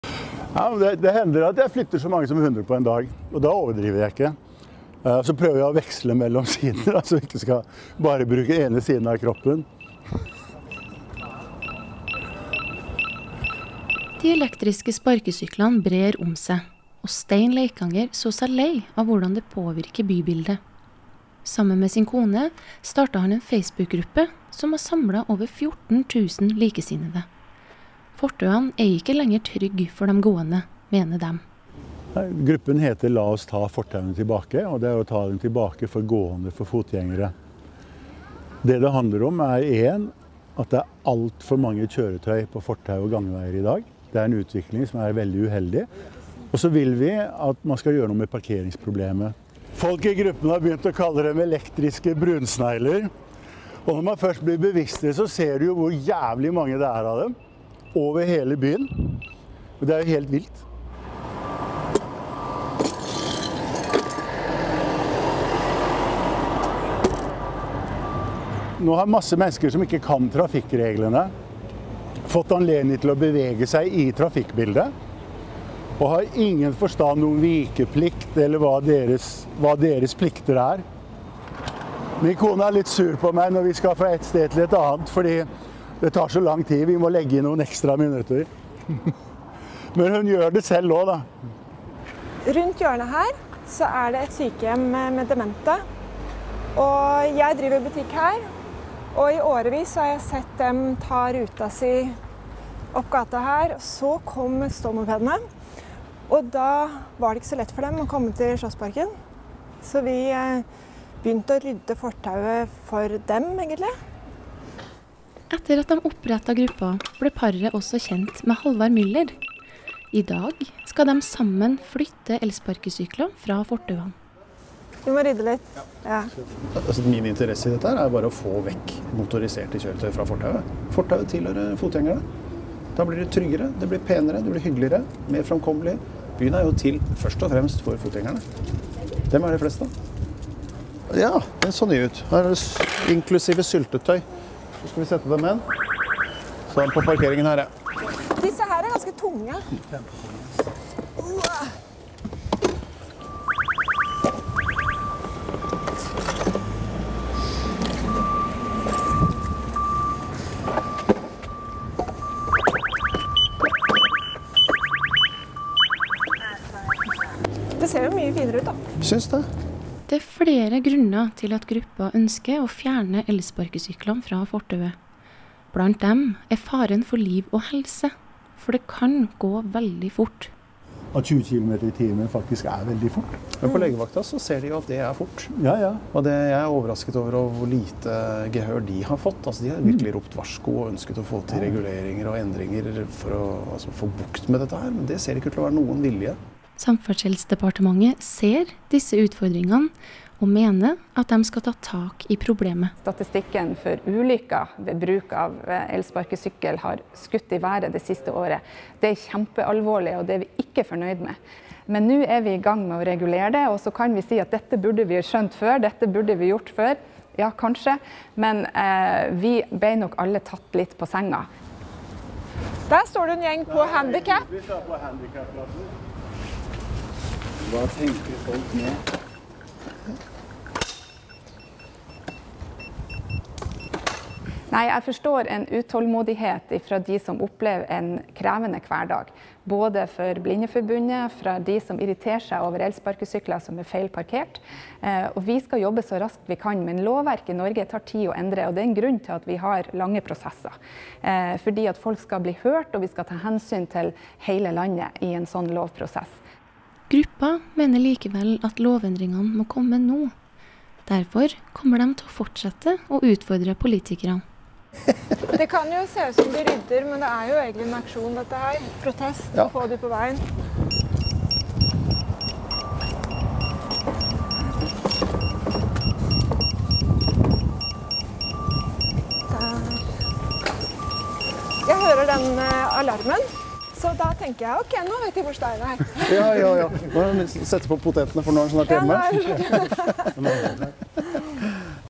De elektriske brunsneglene
El-sparkesyklene sprer seg fort, og flere utrykker bekymring for manglende reguleringer.

Laget av Julie Byberg Bø, Agusta Magnusdottir og Ada Bjøranger